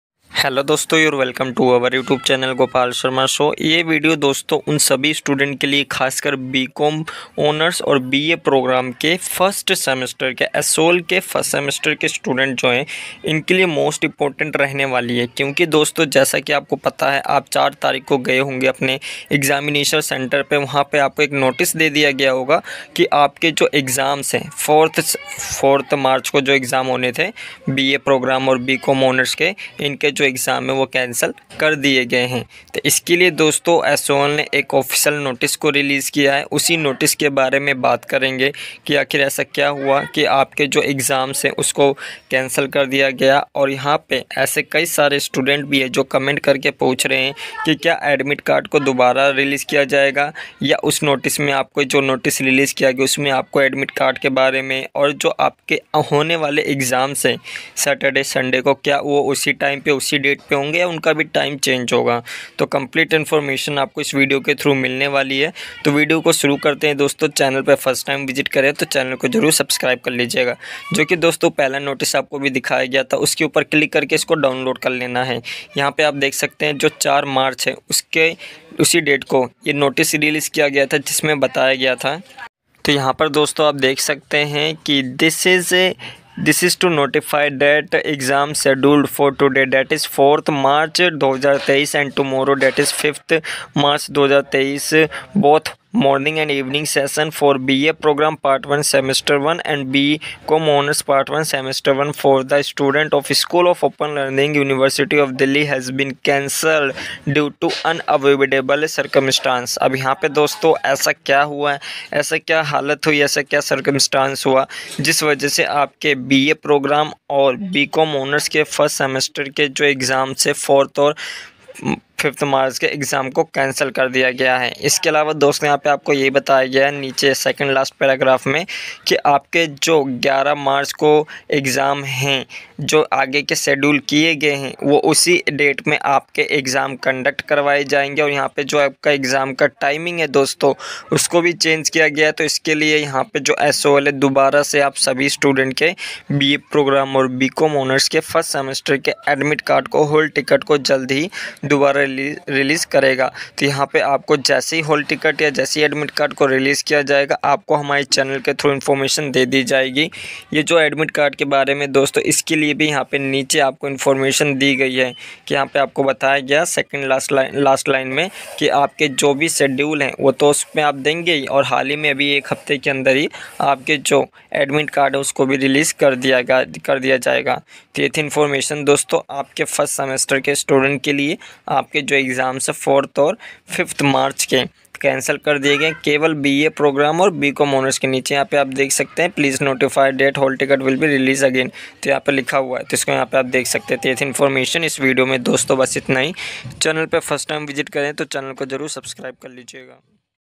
BCom Hons & BA Prog 1st semester Sol Exam Cancelled l Admit कार्ड दोबारा आएगा/Date और time change.
SOL 1st semester BA programme or BCom Hons ke 4th March 5th March ke exam ko cancel kar diya Gaya..

hello everyone..

yes video mein dost aapko bhi formation Di gai hai ki aapke Jo BA programme aur BCom Hons ke student Hain inke Jo 1st semester ke hai 4th aur 5th March ke inke exam ko cancel kar diya gaya hai kya circumstance rahe jiski vajah se aapke exam ko cancel Kiya Gaya..